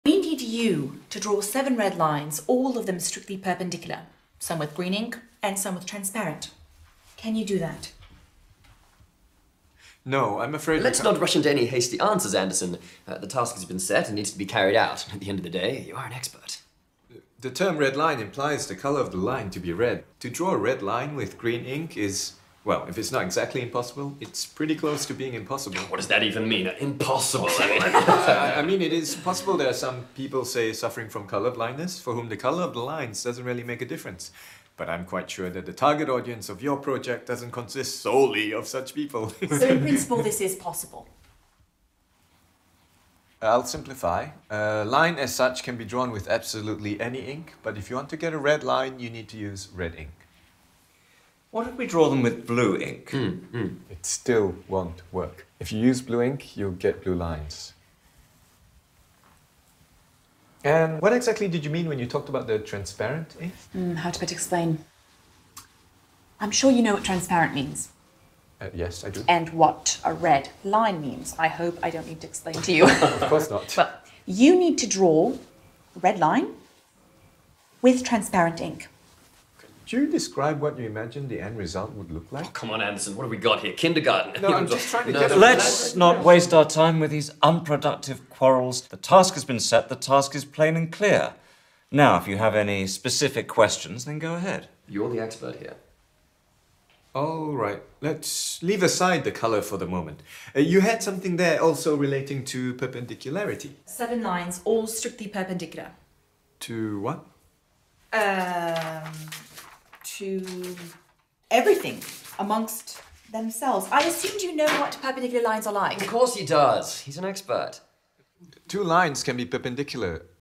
Drawing Perpendicular Red Lines: Impossible Task?
An expert faces a seemingly impossible task: drawing seven strictly perpendicular red lines, some with green ink and some with transparent ink. The expert explores the boundaries of possibility and logic.